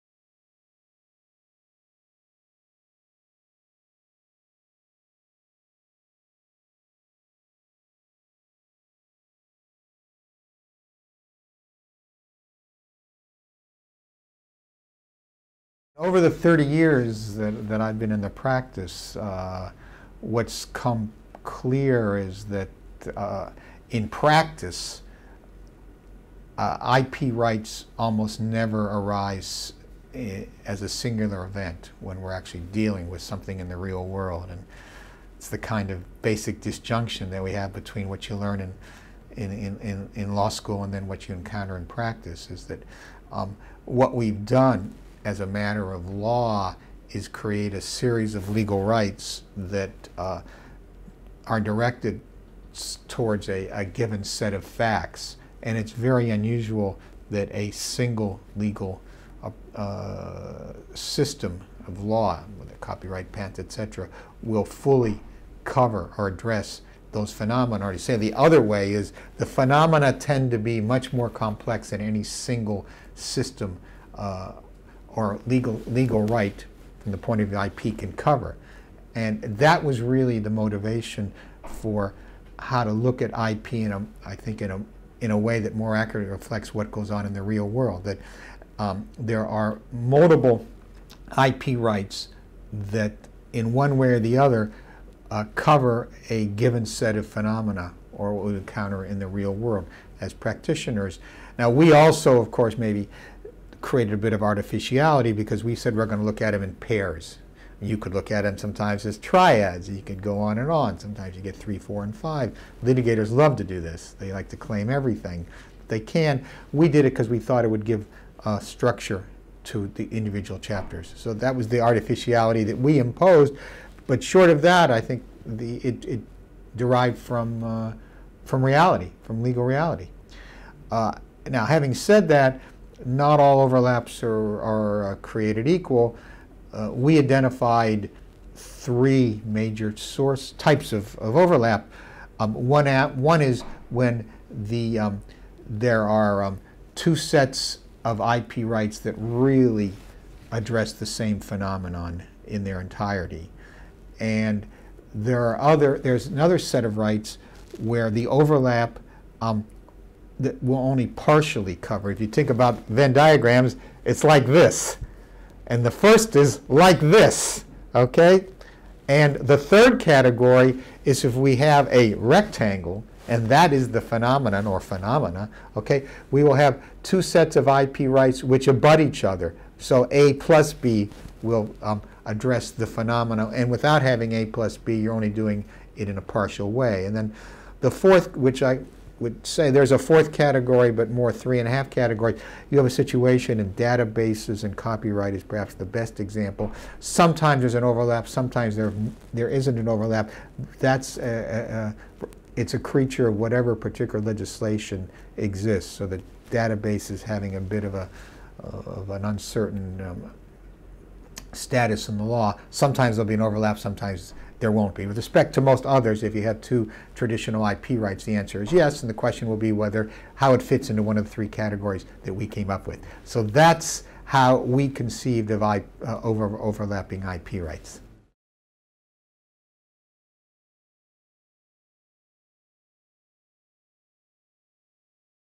An Introduction to Overlapping Intellectual Property Rights
Neil Wilkof talks about what he and Shamnad Basheer hoped to achieve when they wrote their book, Overlapping Intellectual Property rights.

He discusses the impact that blogging has had on the world of intellectual property, and looks forward to how overlapping intellectual property rights might evolve in the future.